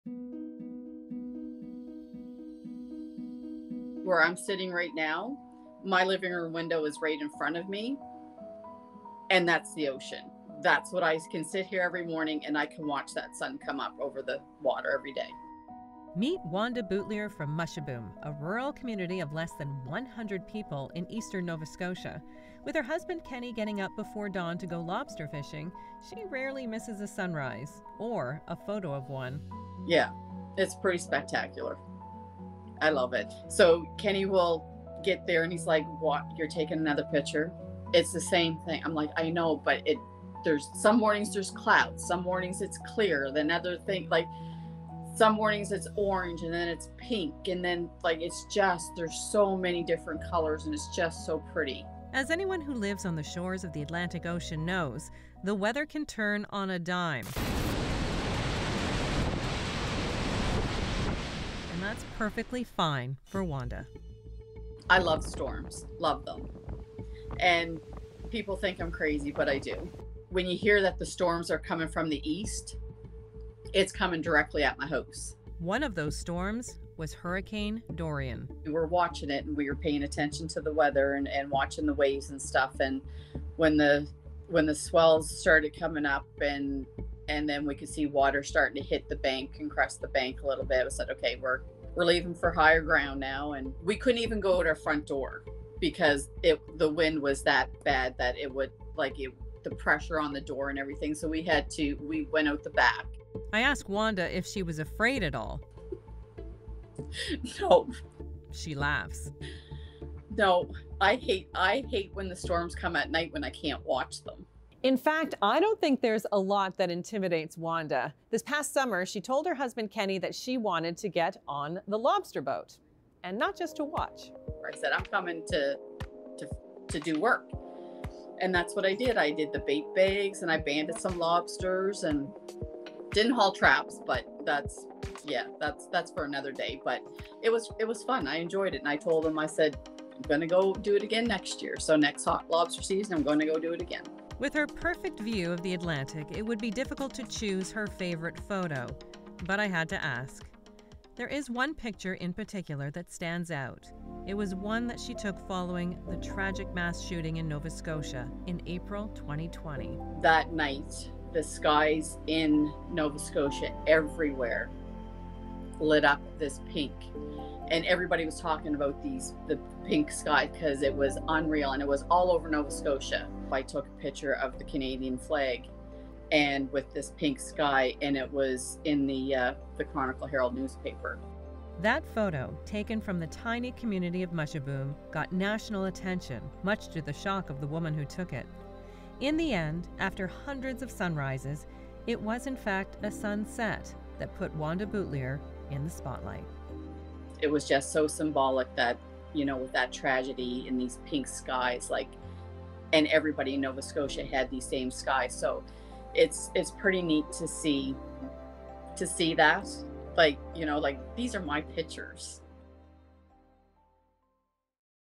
As first light hits Canada, endless colour combos make for powerful photos
Wanda Boutilier of Nova Scotia has a perfect vantage point of the Atlantic and captures photos of the sunrise that never get old.